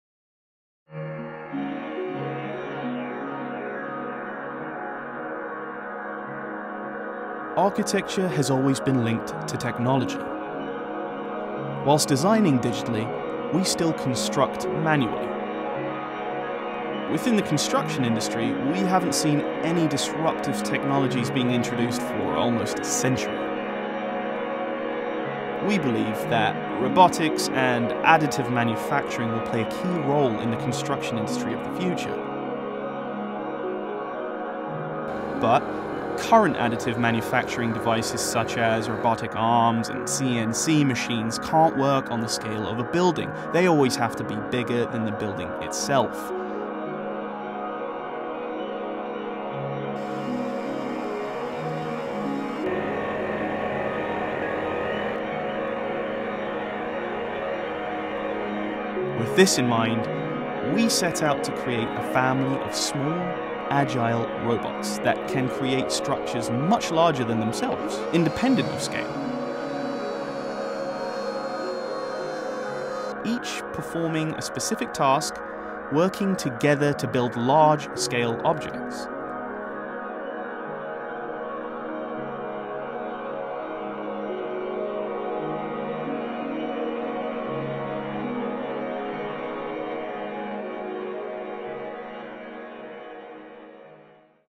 Spanish robot builders
3D-printing has been used in almost all spheres of human activity and, considering what opportunities it opens up, there is nothing surprising that builders and architects are increasingly trying to use it in the construction of buildings. Another attempt made by researchers from the Institute for Advanced Architecture of Catalonia in Barcelona, ​​is specially designed robots Minibuilder, equipped with feeder liquid cement. These devices, in spite of its small size, are able to create quite a bulky design, the truth has the same drawback as 3D-printers: a fairly low-speed printing. However, further research in this area do not stop and it is possible that very soon we will see buildings constructed without direct human intervention.